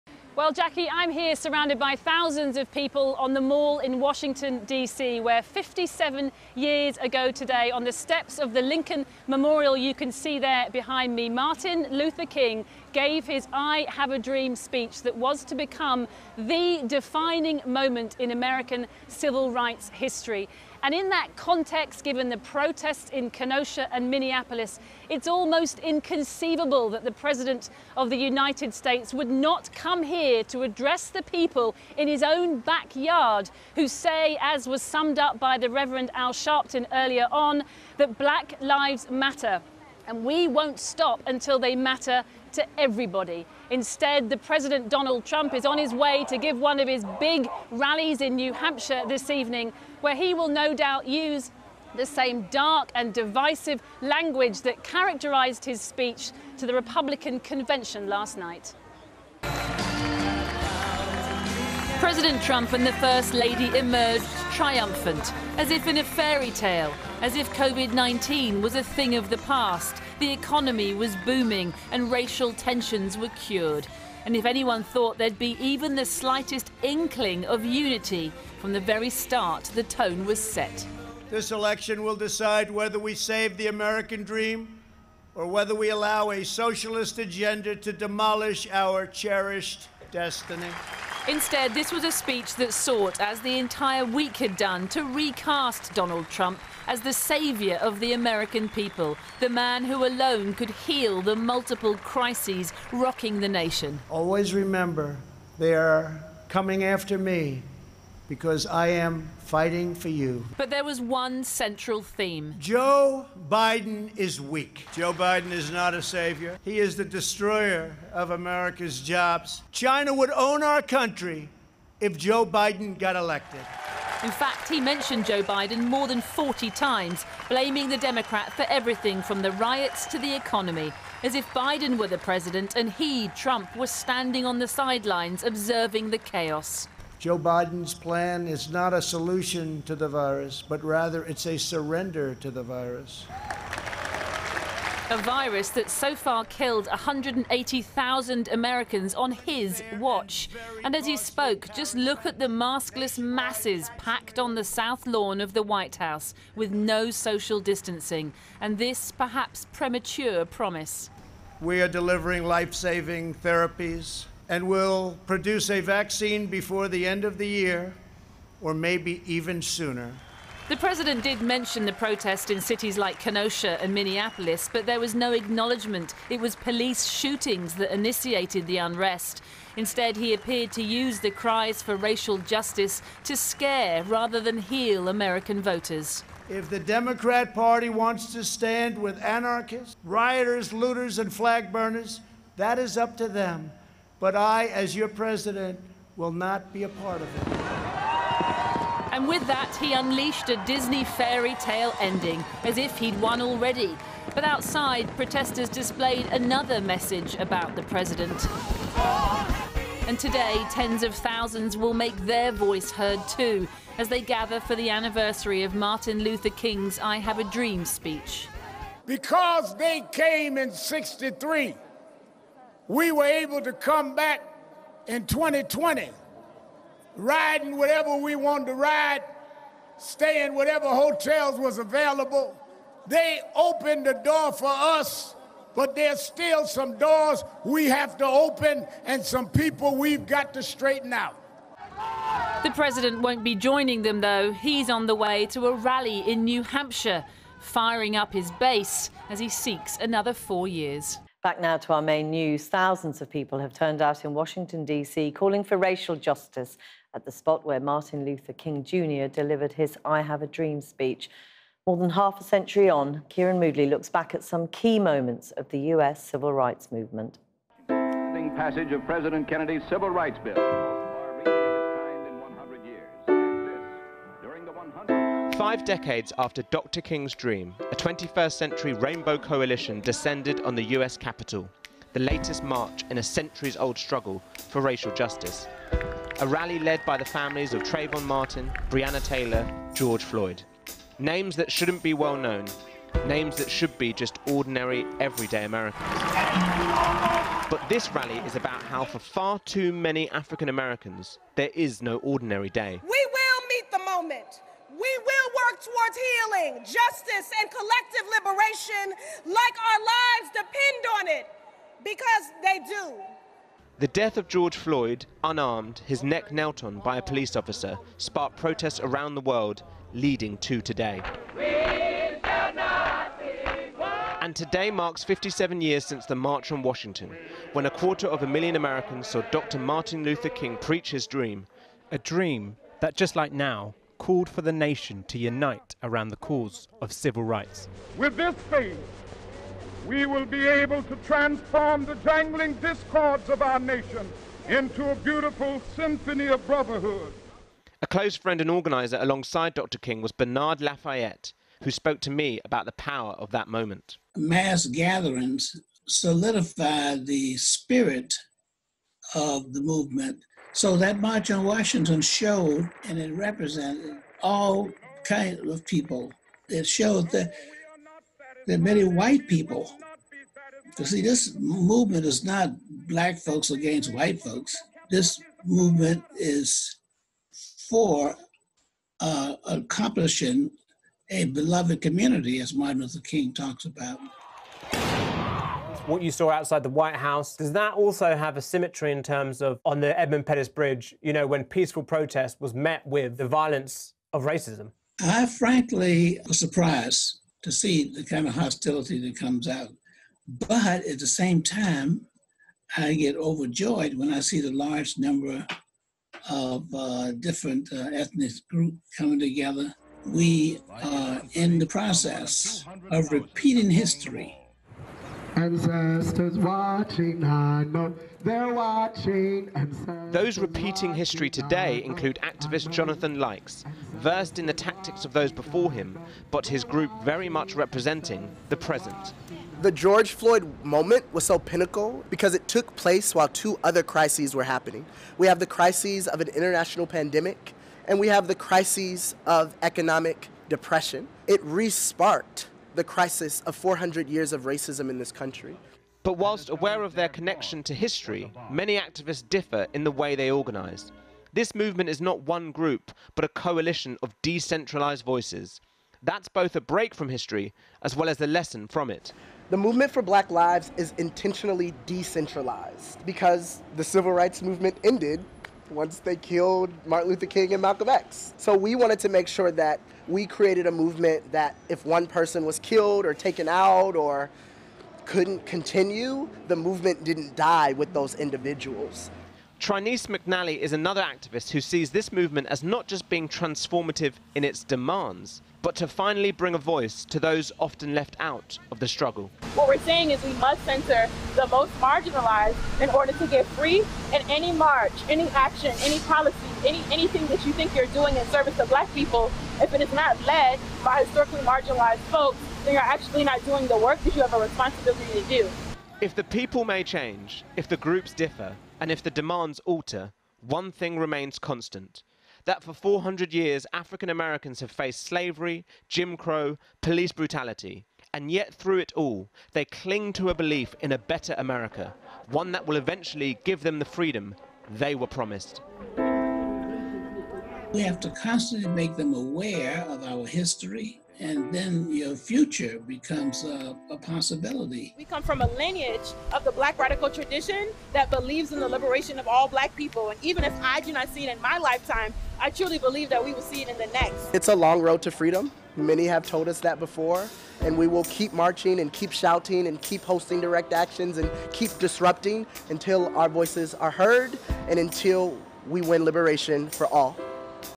Thousands gather to commemorate anniversary of Martin Luther King’s I Have a Dream speech
Thousands of people calling for racial justice have gathered near the steps of the Lincoln Memorial in Washington DC - more than half a century after Martin Luther King Jr's historic speech - and amid yet another week of protests against police and vigilante violence. (Subscribe

Relatives of Black Americans shot or killed by police are expected to speak to the crowds, along with civil rights leaders, at what could be the biggest political gathering in Washington since the pandemic began.